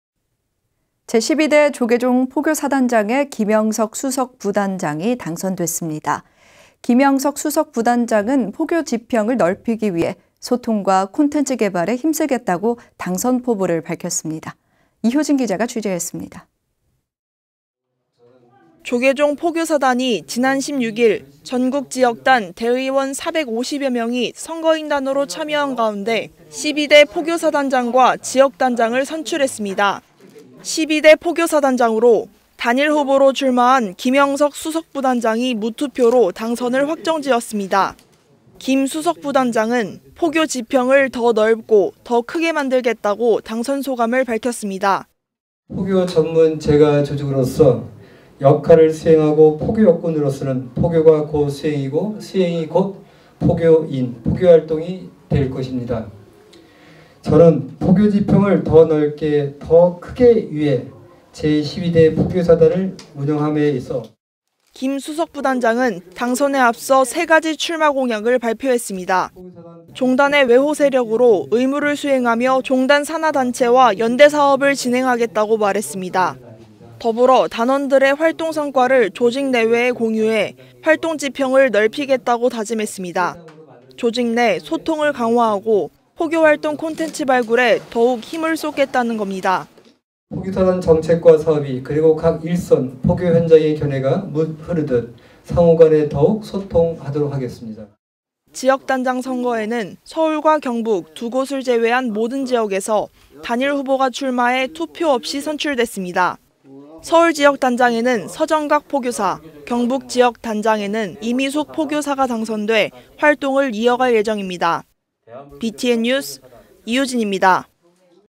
[BTN뉴스] 조계종 포교사단, 김영석 제12대 단장 선출
〔앵커〕

제12대 조계종 포교사단장에 김영석 수석부단장이 당선됐습니다. 김영석 수석부단장은 포교지평을 넓히기 위해 소통과 콘텐츠 개발에 힘쓰겠다고 당선 포부를 밝혔습니다. 이효진 기자가 취재했습니다.
〔리포트〕

조계종 포교사단이 지난 16일, 전국 지역단 대의원 450여 명이 선거인단으로 참여한 가운데, 12대 포교사단장과 지역단장을 선출했습니다.

12대 포교사단장으로 단일후보로 출마한 김영석 수석부단장이 무투표로 당선을 확정지었습니다.

김 수석부단장은 포교 지평을 더 넓고 더 크게 만들겠다고 당선소감을 밝혔습니다.

김영석 / 조계종 제12대 포교사단장
(포교 전문 재가 조직으로서 역할을 수행하고, 포교 역군으로서는 포교가 곧 수행이고, 수행이 곧 포교인 포교활동이 될 것입니다. 저는 포교지평을 더 넓게, 더 크게 하기 위해 제12대 포교사단을 운영함에 있어 (최선을 다할 것입니다.))

김 수석부단장은 당선에 앞서 세 가지 출마공약을 발표했습니다.

종단의 외호세력으로 의무를 수행하며 종단 산하단체와 연대 사업을 진행하겠다고 말했습니다.

더불어 단원들의 활동성과를 조직 내외에 공유해 활동 지평을 넓히겠다고 다짐했습니다.

조직 내 소통을 강화하고, 포교활동 콘텐츠 발굴에 더욱 힘을 쏟겠다는 겁니다.

(포교단원 정책과 사업이, 그리고 각 일선 포교현장의 견해가 물 흐르듯, 상호간에 더욱 소통하도록 하겠습니다.)

지역단장선거에는 서울과 경북 두 곳을 제외한 모든 지역에서 단일후보가 출마해 투표없이 선출됐습니다.

서울 지역단장에는 서정각 포교사, 경북 지역단장에는 이미숙 포교사가 당선돼 활동을 이어갈 예정입니다.

BTN 뉴스 이효진입니다.